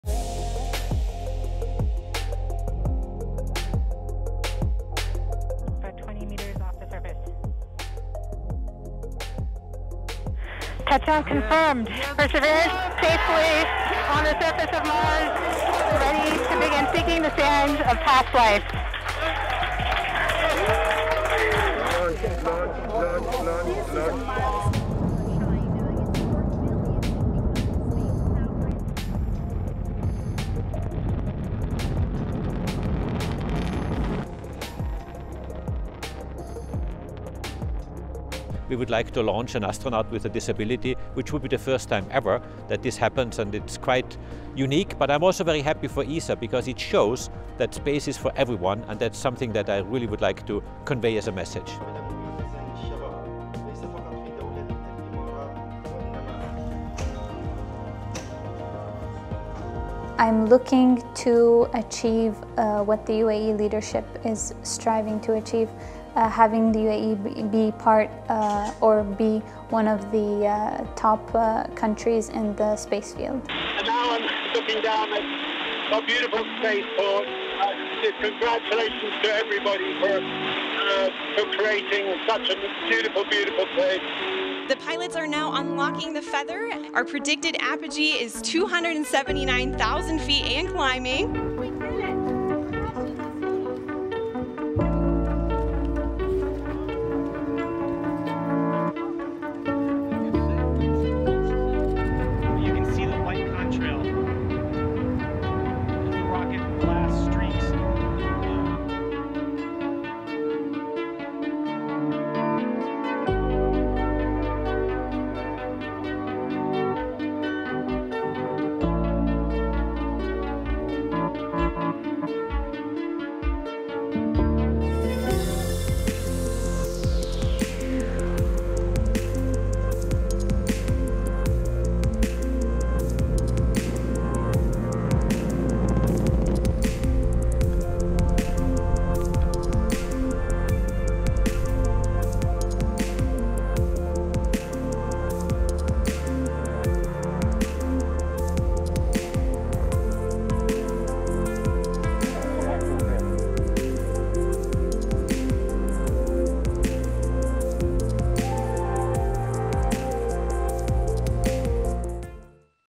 2021’s space exploration highlights
NASA's Mars landing, billionaires in space, first Arab woman astronaut and more – here are some of the highlights in space exploration this year.